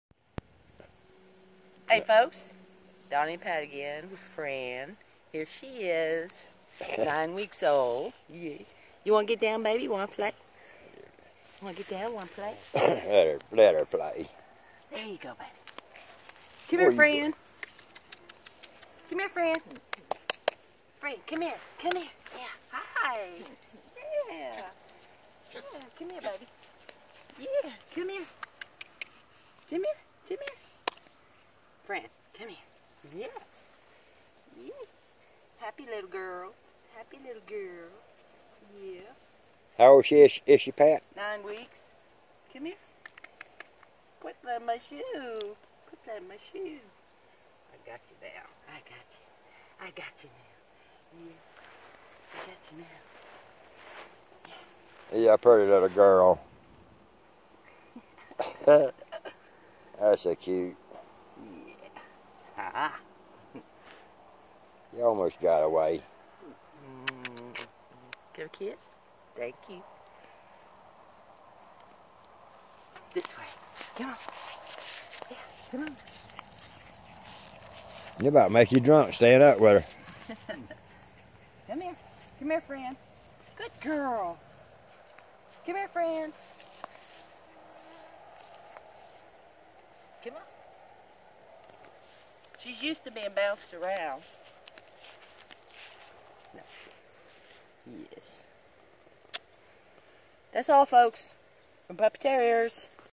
Fran with Patricia & Donny @ Puppyterriers.com
Georgia is so Hot & humid right now, we did a short video outside with Available Fran. HAPPY 4TH Y'ALL! 🐶🇺🇸😊😎🛣🛶🚣‍♀️🐕